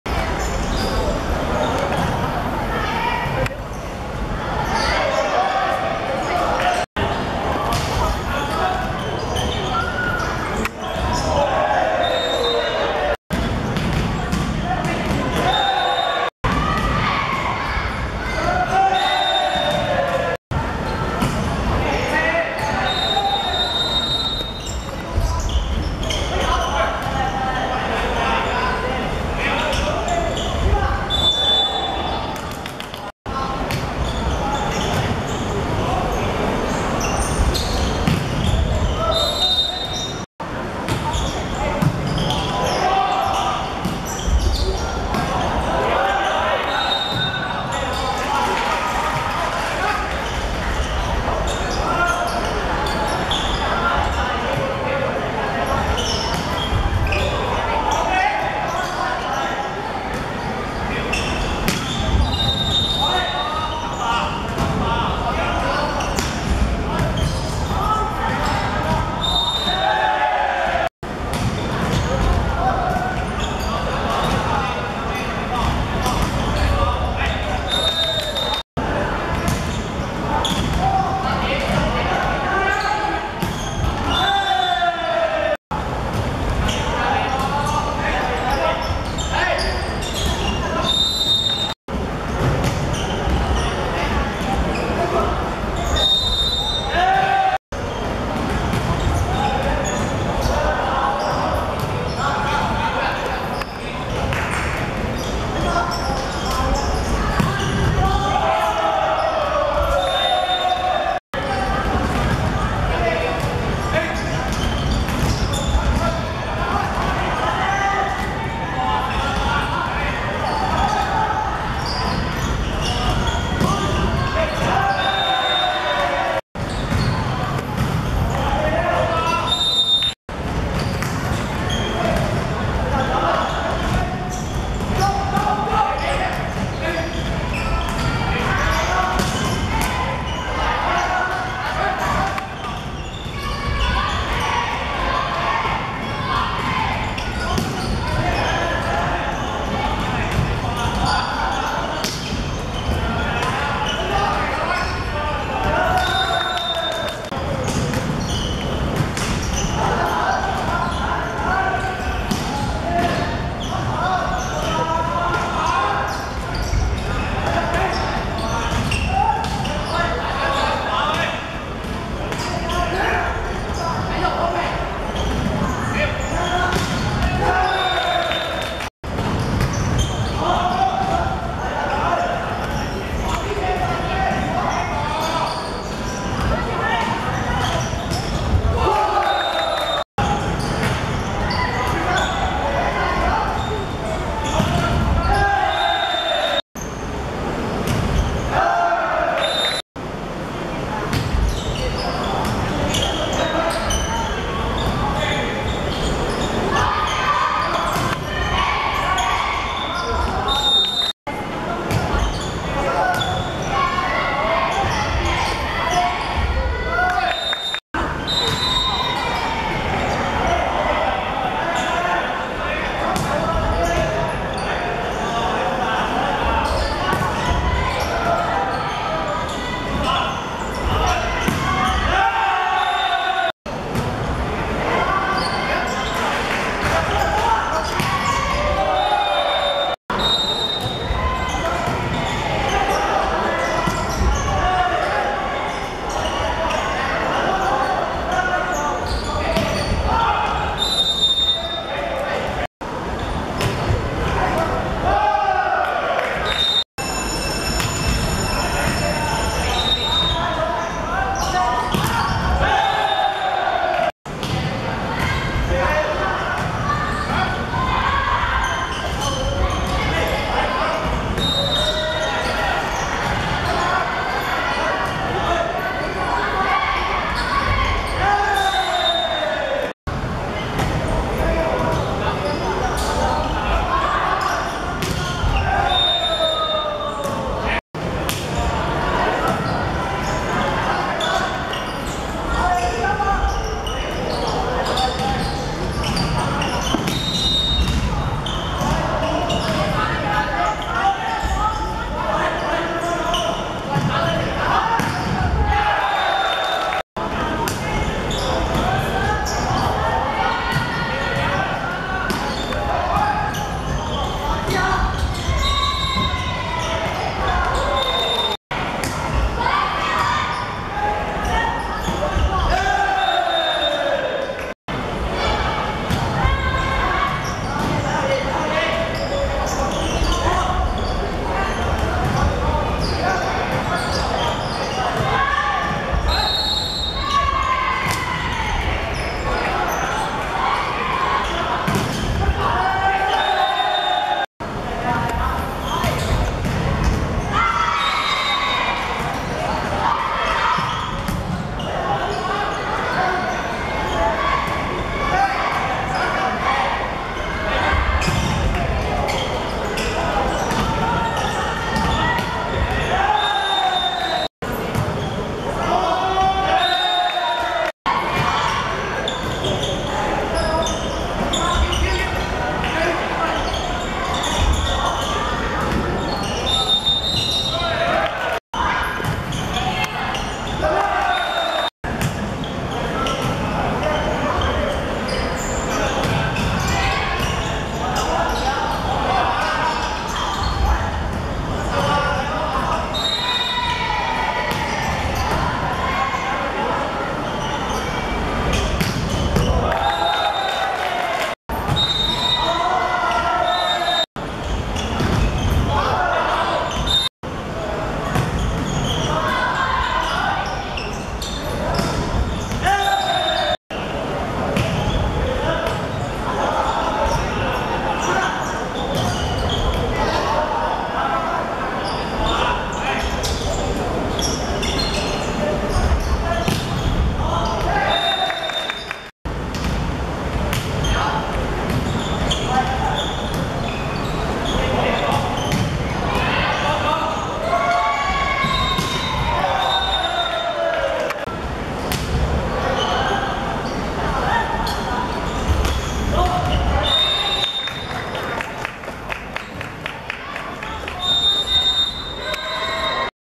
培英vs伊中舊生會(2015.12.27.學界排球精英賽男子複賽)第一局精華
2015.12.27.青衣體育館 16:17 第一局賽果 培英(綠) 25 : 17 伊中舊生會(白)